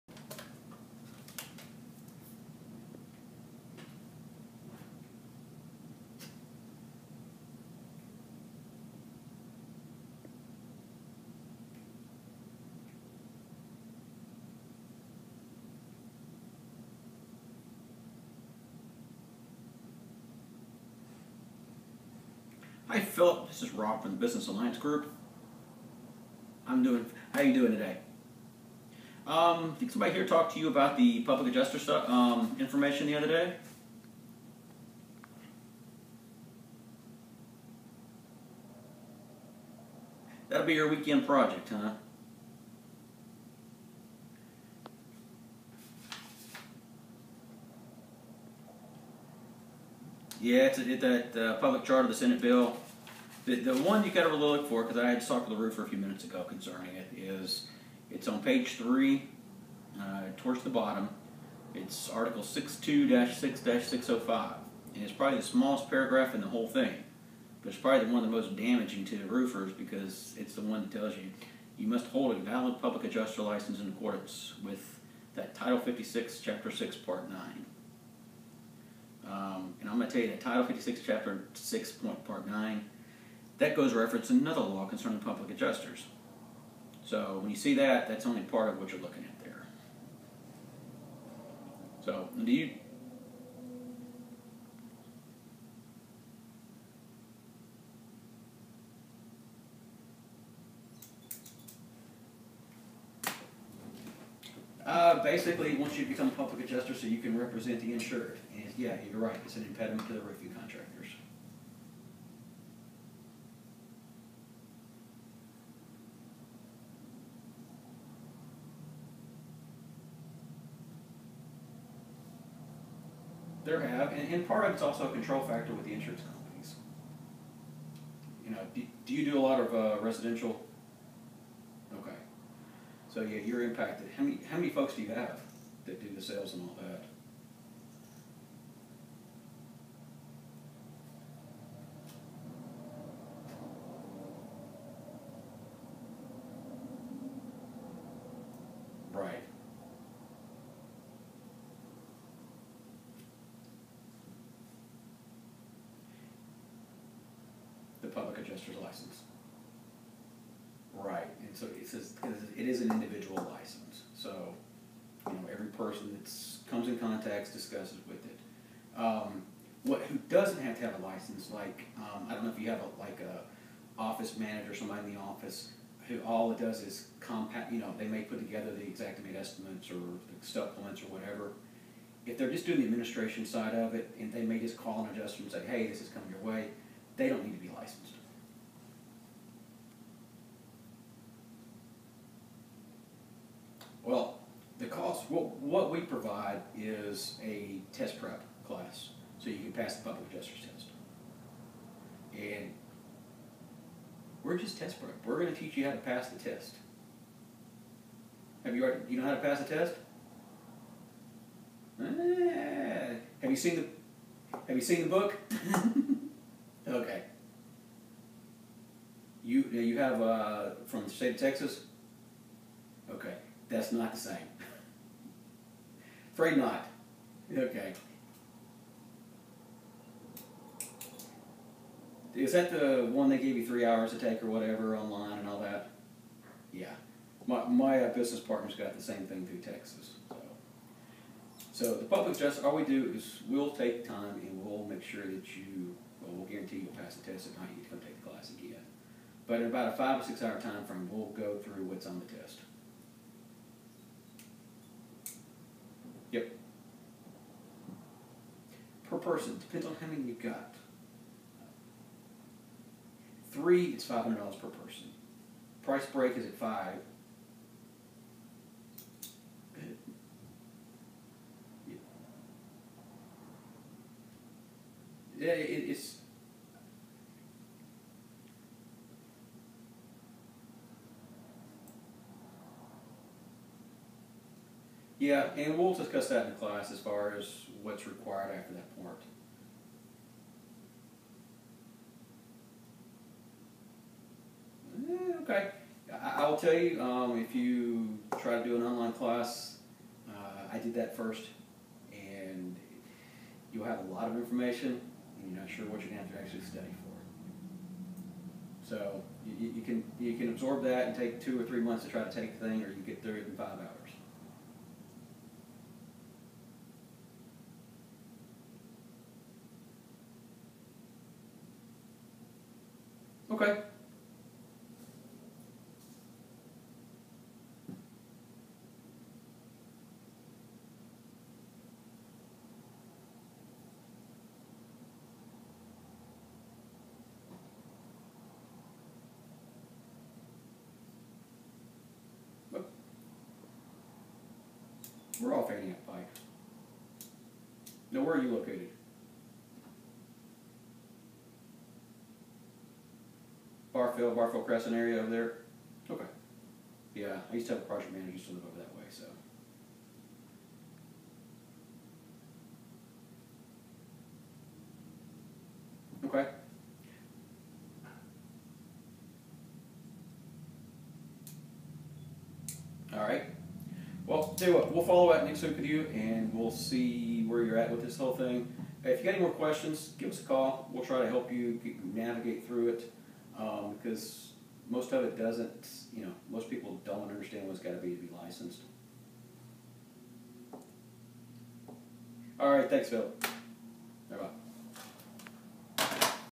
How to Overcome Objections 1.MOV
Rob Drexler, experienced Public Adjuster, calls a roofer and answers questions about the new Public Adjuster law and our classes. Listen to how he overcomes the caller's objections. How might you overcome similar objections?